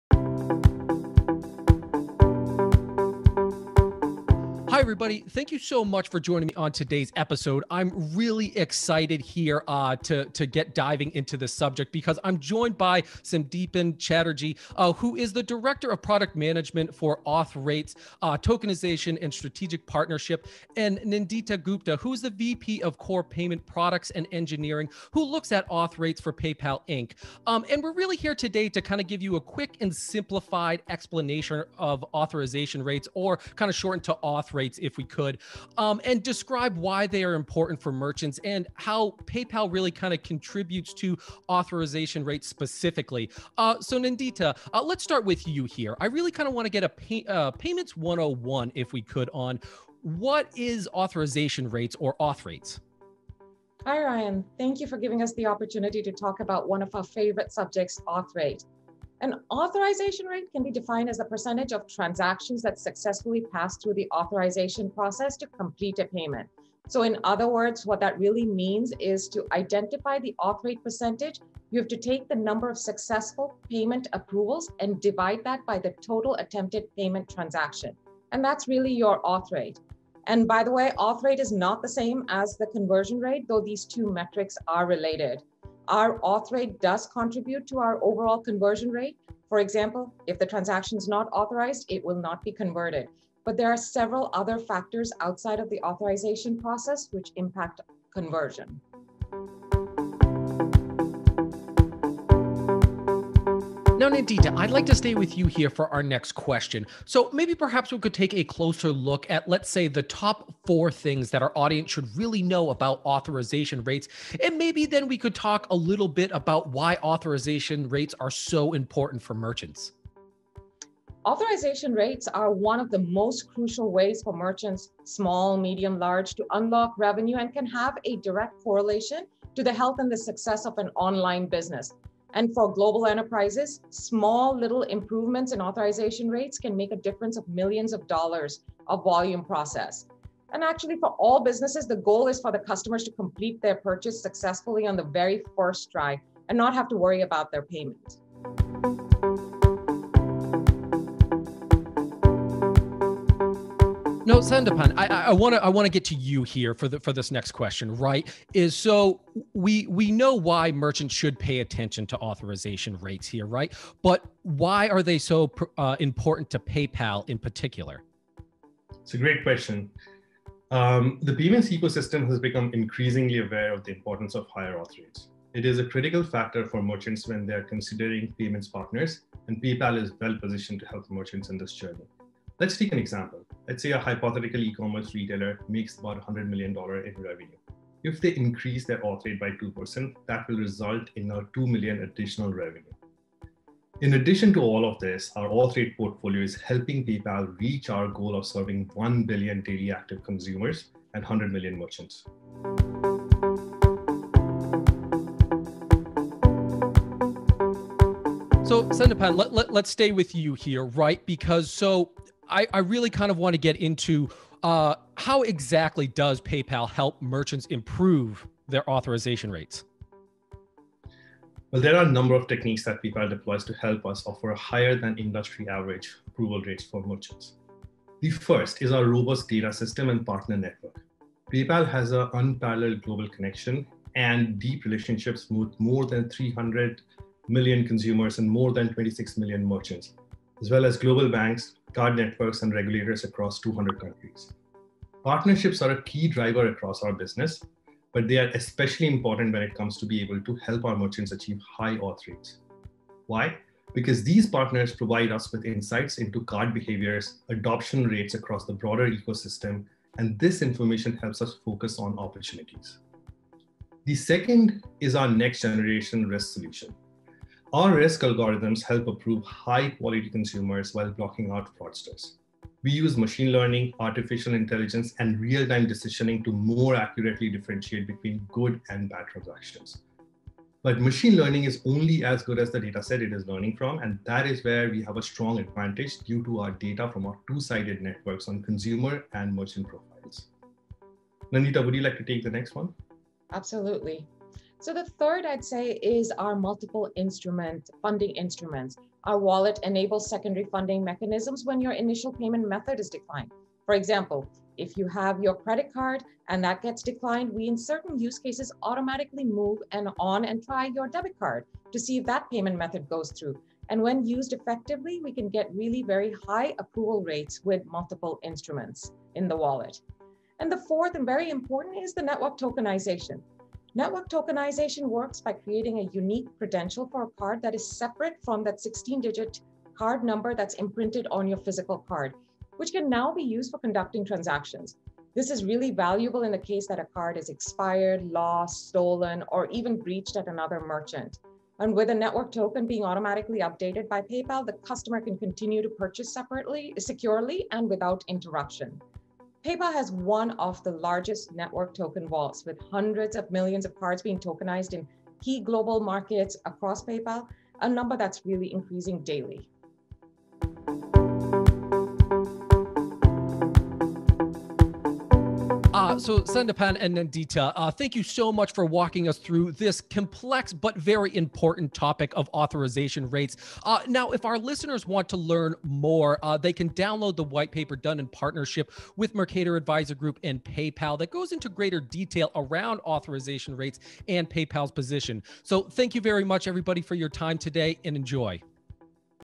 What Are Authorization Rates and Why Are They Important to Merchants?
Every time a customer swipes, inserts, or taps their debit or credit card, it requires an authorization. A successful authorization implies that a bank account or line of credit has sufficient attainable value that can be set aside for a purchase until it is fully processed and the transaction is complete.
If there are not enough funds available to complete the purchase, then the transaction will be declined. The rate of successful transactions help to calculate the authorization rate, or auth rate. But what are auth rates, and why should merchants care about them?

To further discuss this payments industry hot topic, PaymentsJournal sat down with PayPal’s Nandita Gupta, VP of Core Payments Products & Engineering, and Sandipan Chatterjee, Director of Product Management for Auth Rate, Tokenization & Strategic Partnerships.